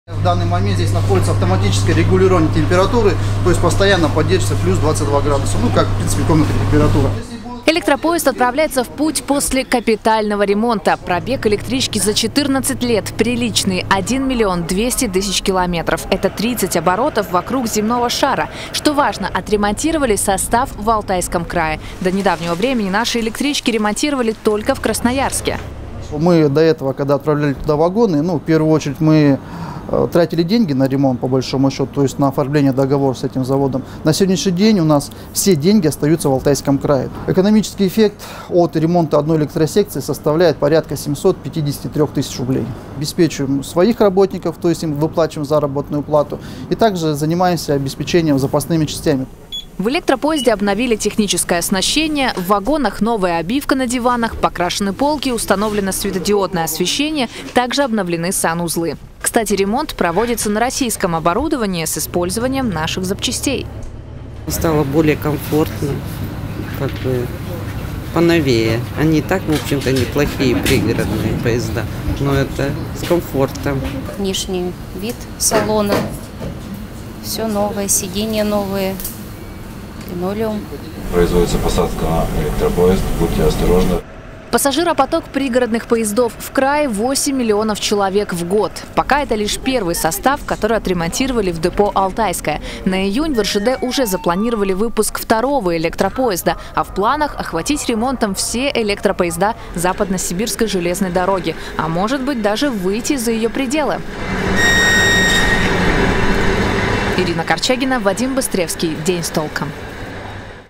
В Алтайском крае начали проводить капитальный ремонт электропоездов
В Алтайском крае начали проводить капитальный ремонт электропоездов. В течение долгого времени наши электрички увозили на ремонт в Красноярский край, в то время как в Алтайском крае проводили лишь текущий ремонт. Наша съёмочная группа своими глазами увидела, как теперь выглядят пригородные поезда нового формата